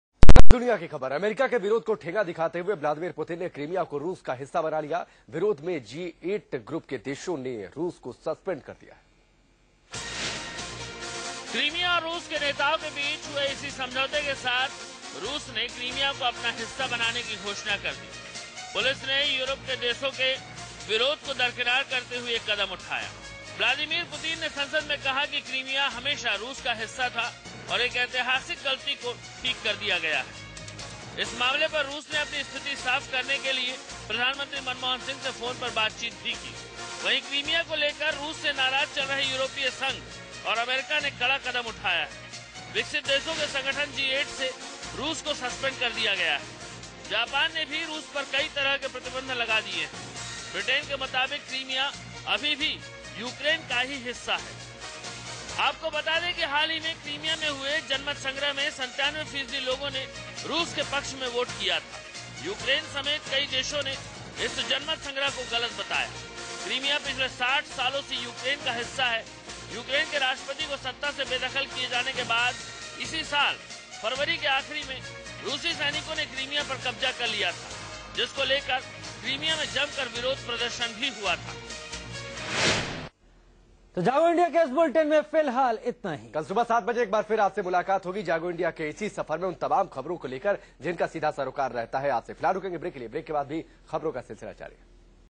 Treaty signed on Crimea's accession to Russia
Russian President Vladimir Putin on Tuesday signed a treaty on Crimea's accession to Russia after delivering a defiant speech defending his move and blasting the West for pursuing "containment" of Russia and flouting international law. The treaty was also signed by the Crimean leaders who attended a special joint session of the Russian Parliament in the Kremlin. Mr. Putin said he was confident the Russian Parliament would ratify the pact.